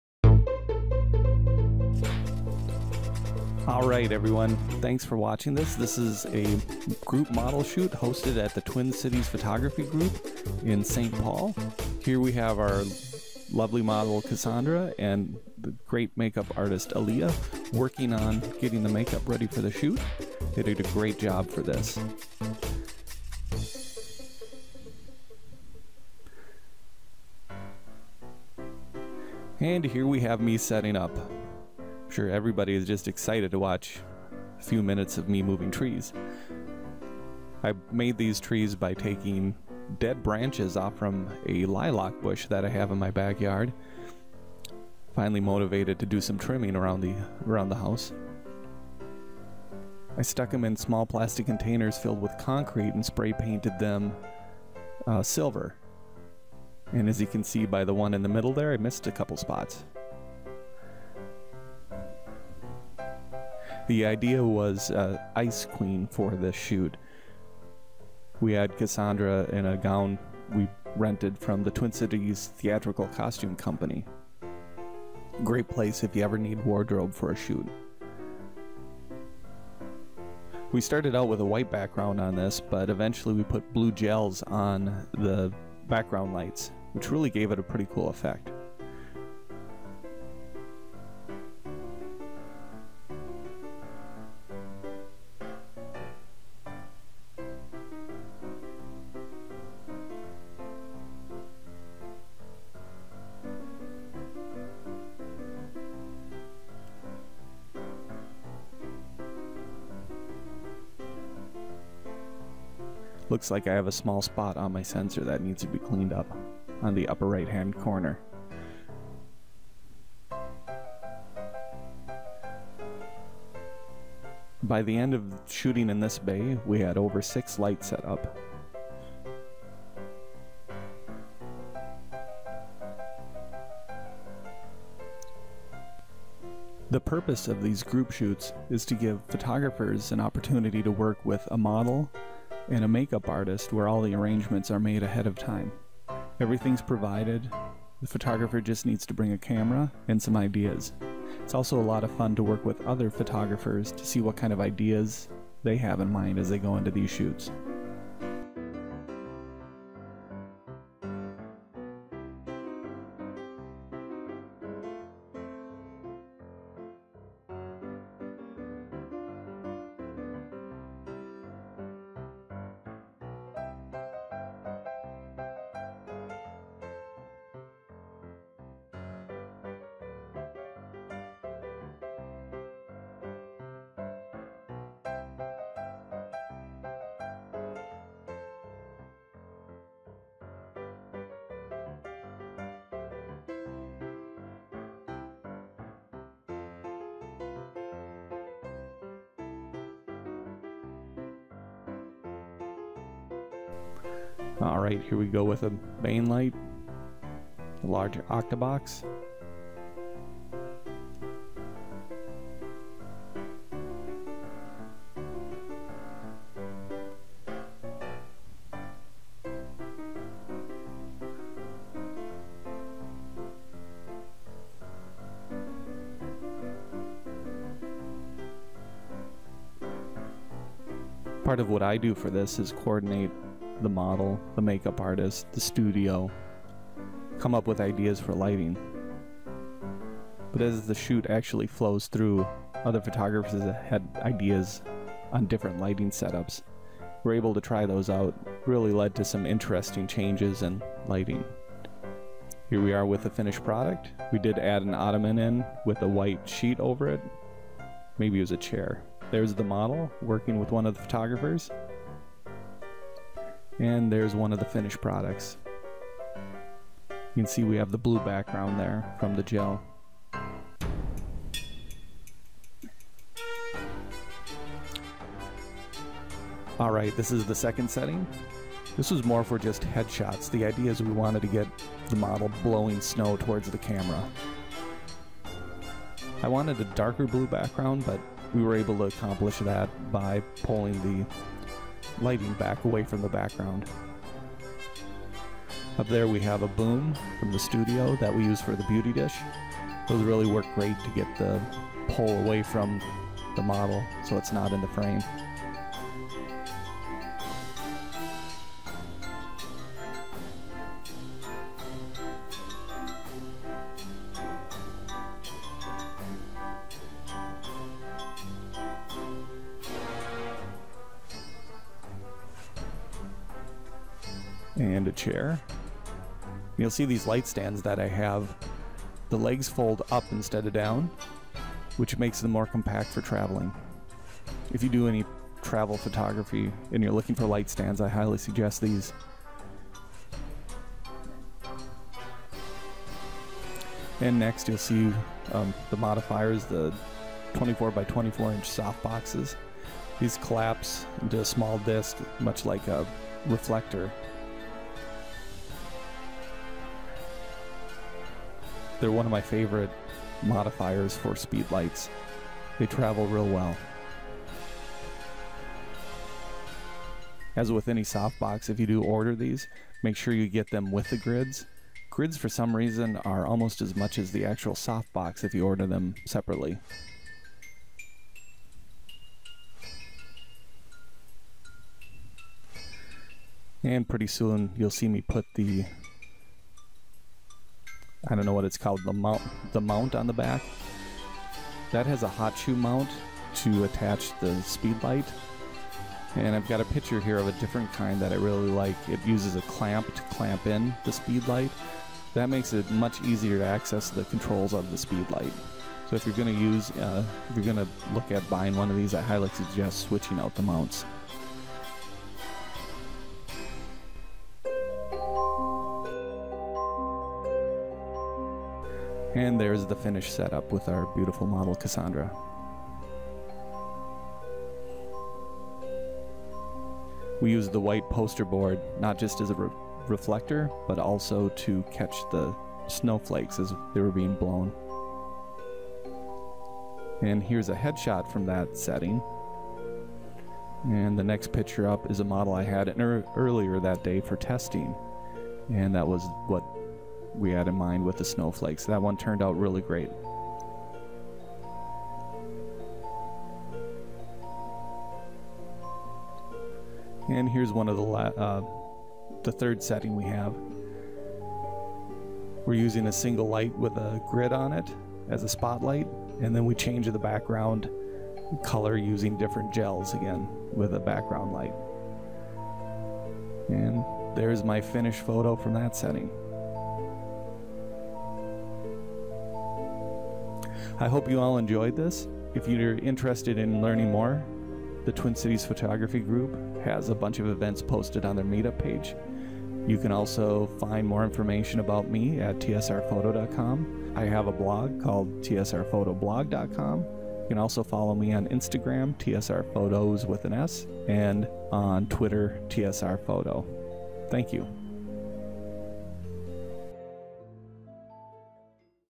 Ice Queen
Behind the scenes Ice Queen themed photo shoot at the Twin Cities Photography Group studio in St. Paul, Minnesota.